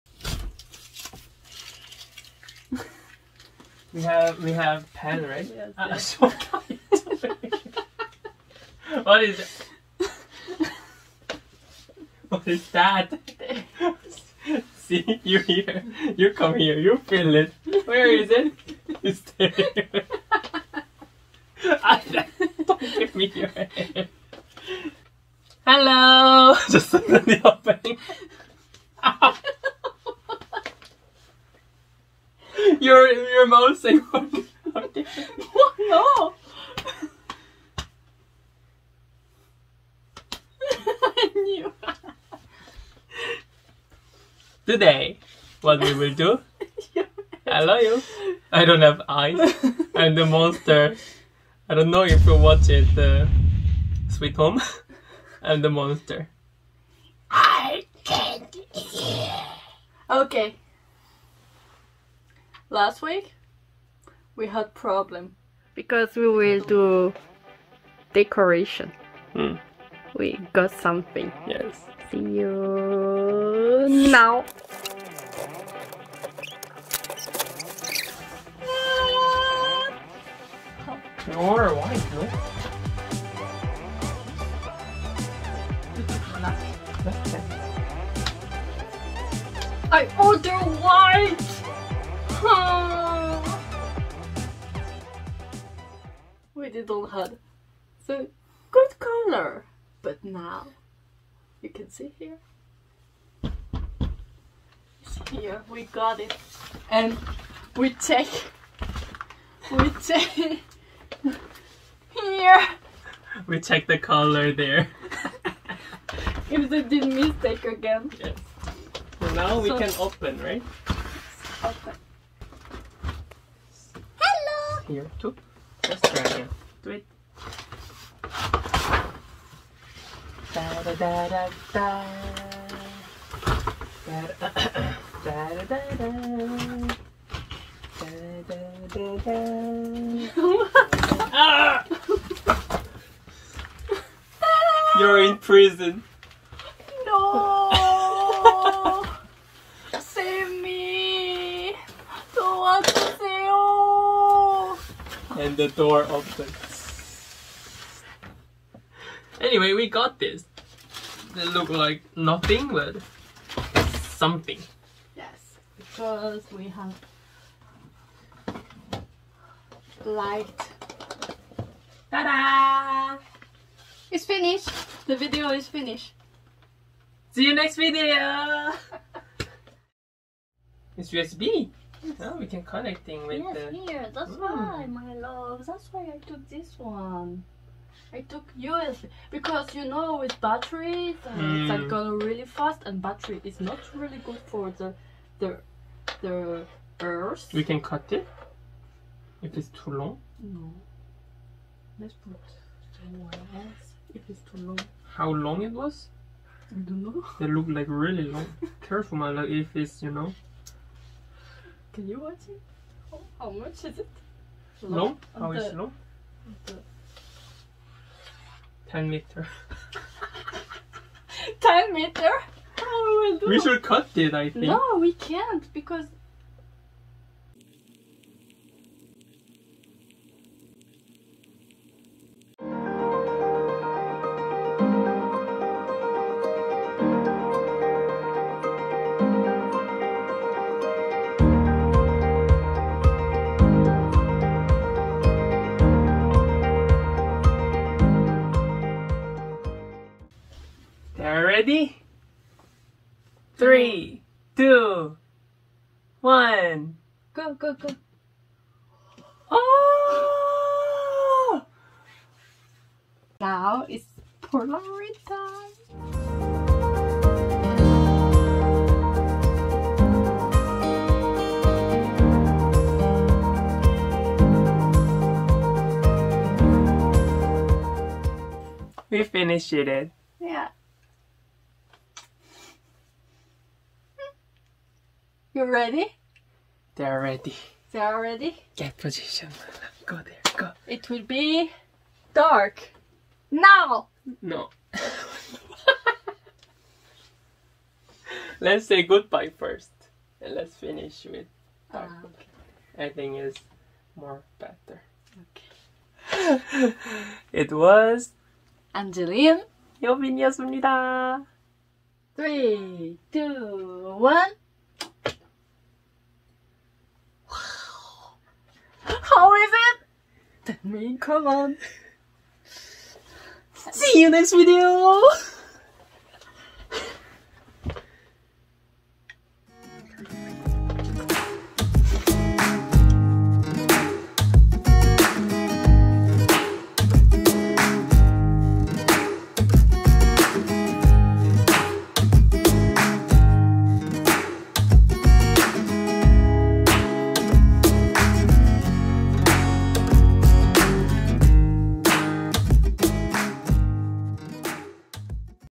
WE DECORATE OUR ROOM in Seoul [Amwf International Couple/국제커플]
Our room was too minimalist so we decided to decorate !